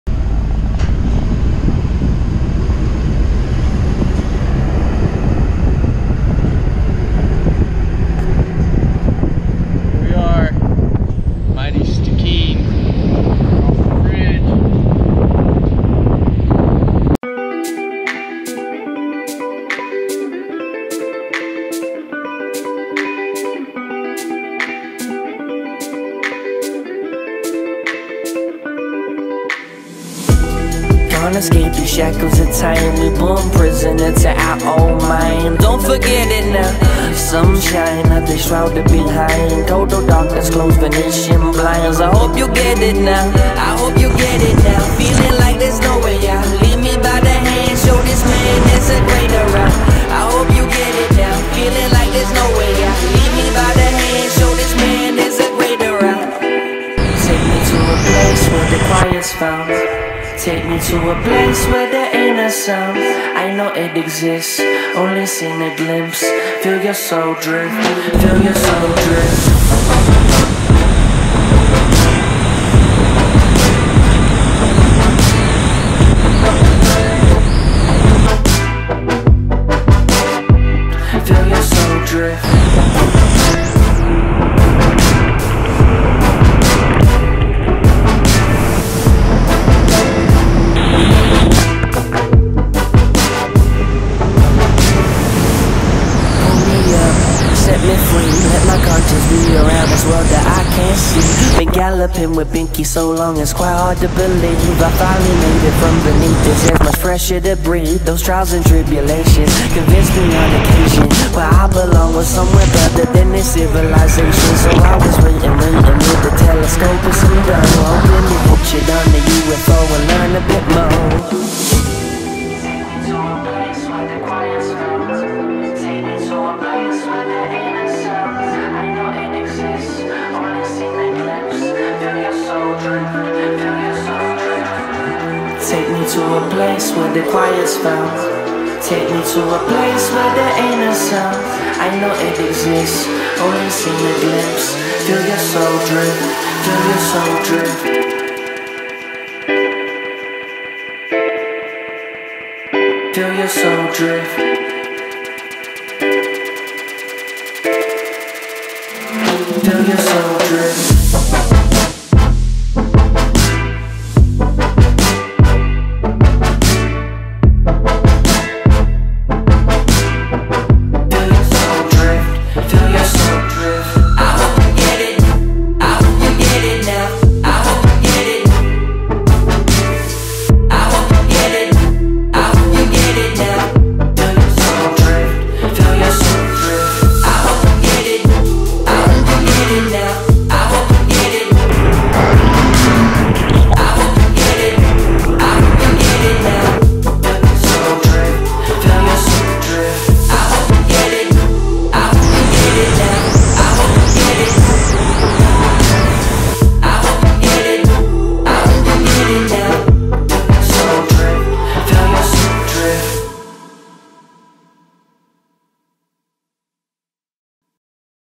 Stikine 2018!
The Stikine has been a dream of mine for a long time and to finally be there at the put-in was surreal. Here are a couple clips from the big water paradise!